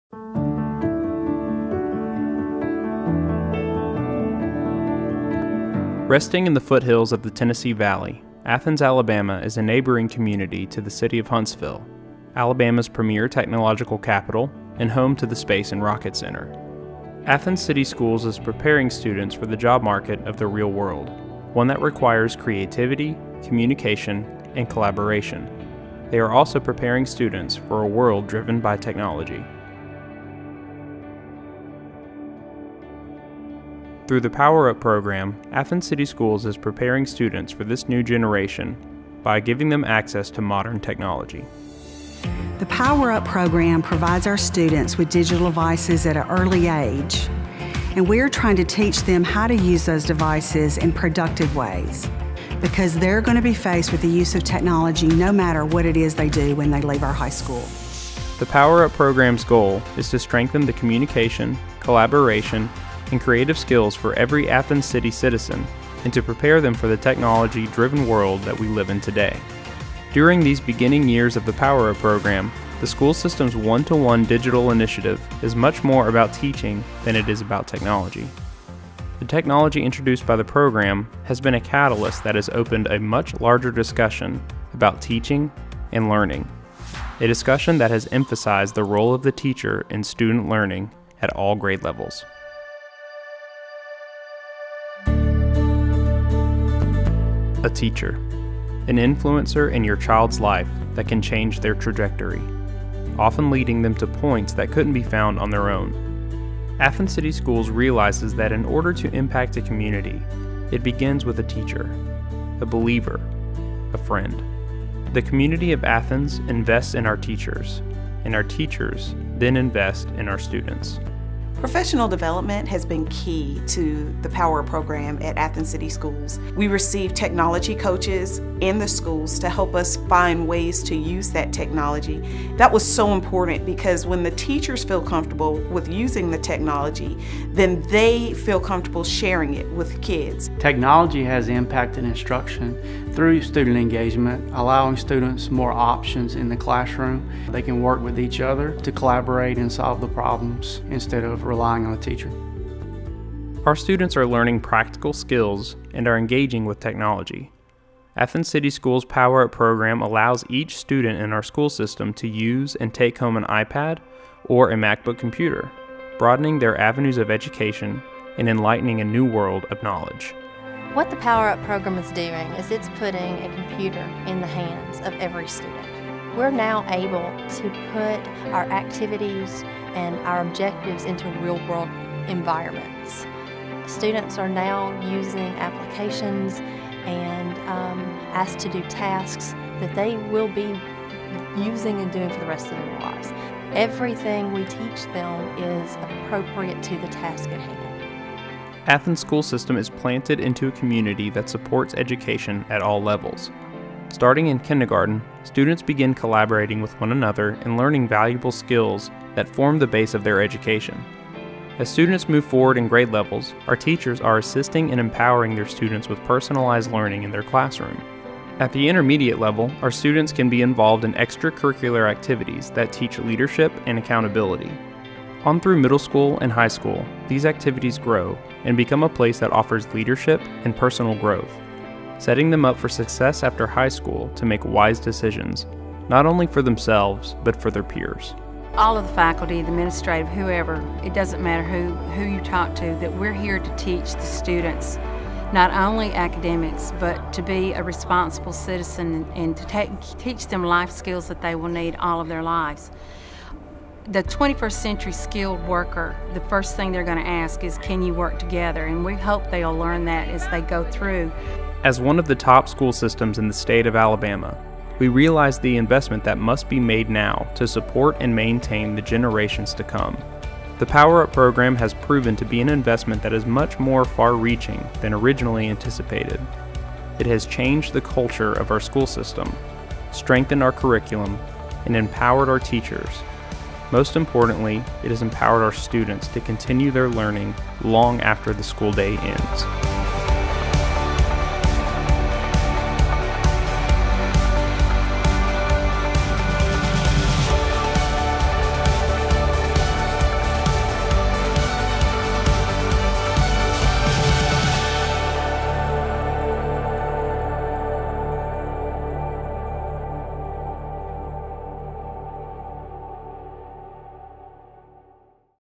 Athens City Schools Power Up Digital Initiative Award Video
Students live in an on-demand, technology-dependent world. They learn differently and approach schoolwork differently than students did even a few years ago.  The Athens City School District is meeting the needs of this new generation of learners by increasing student access to computer technology during the school day.

The PowerUP Program allows the Athens City School District to provide a challenging, dynamic academic program that integrates the best of traditional education with new and emerging technologies.  Creative teaching strategies combined with engaging technology tools empower students and teachers to reach the highest standards of educational excellence. Rather than teaching technology as a discrete subject, the Athens City School District uses a holistic approach where the technology skills and related computer projects are integrated throughout the daily curriculum to foster active, student-centered, hands-on learning.  The goal of our PowerUP Program is to strengthen the communication, collaboration, and creative ability for all students in all classes.